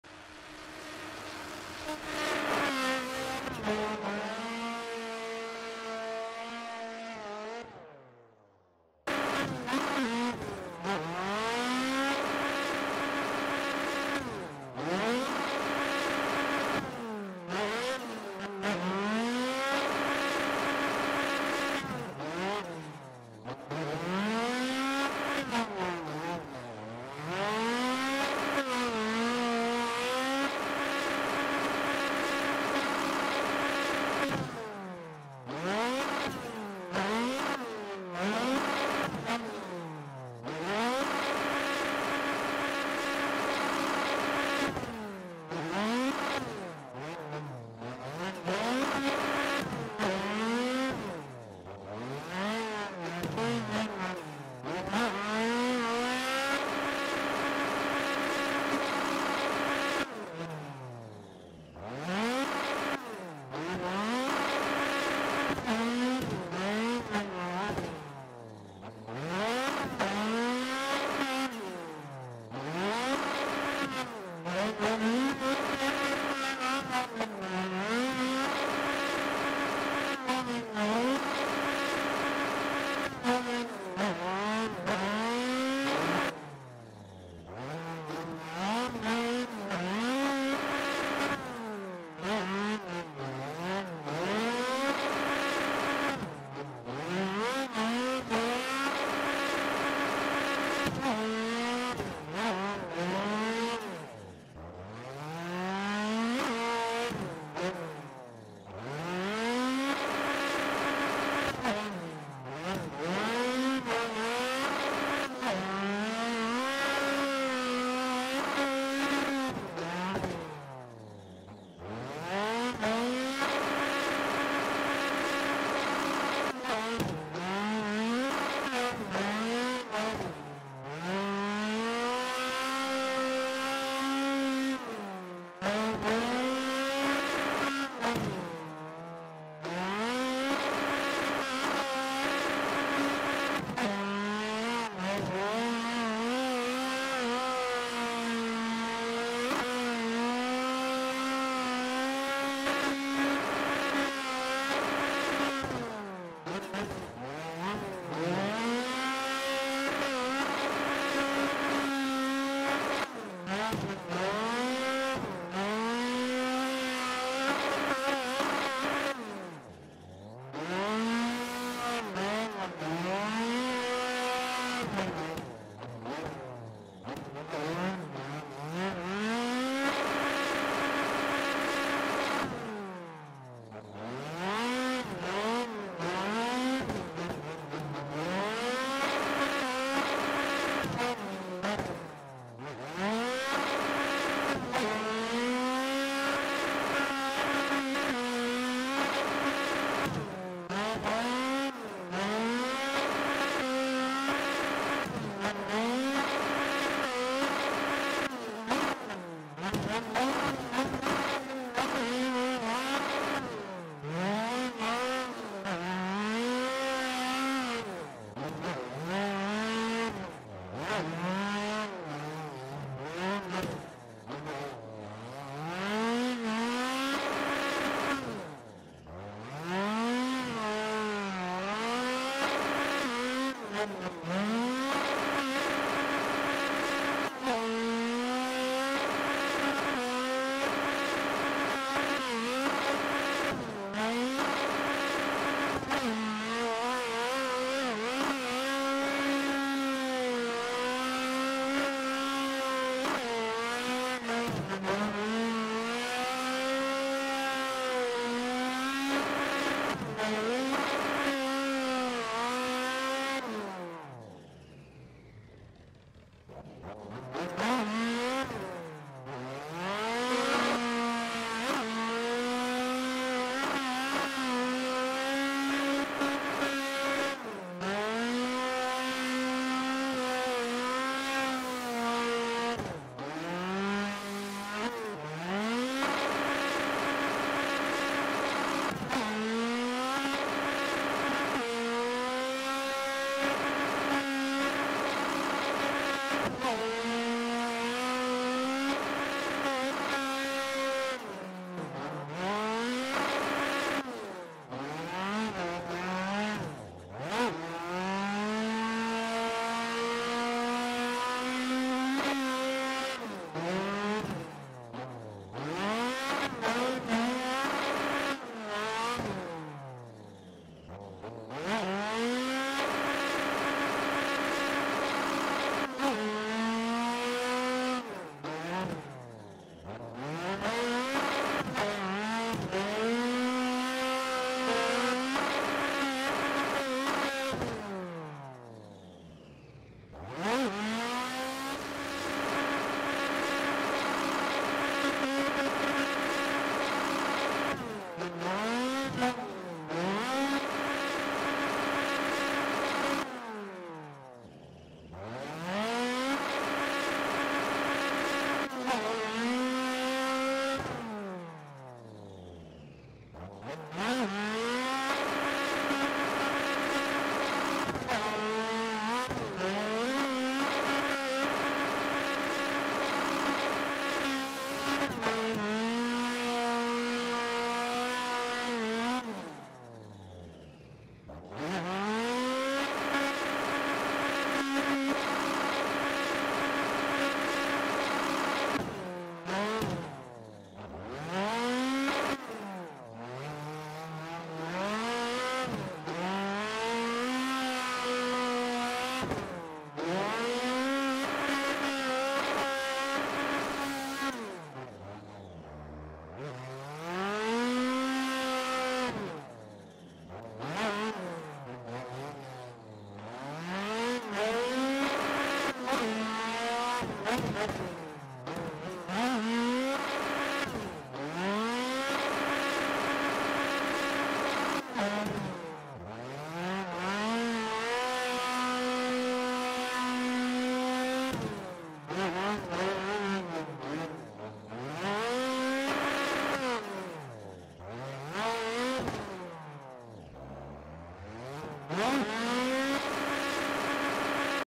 Nissan 200sx drifting THE GTA 5 MAP - MOZA R5
Nissan 200sx drifting THE GTA 5 MAP - MOZA R5 gameplay

Ready to take your racing to the next level?

pc spec:
i7 8700
msi 2070
RAM: 16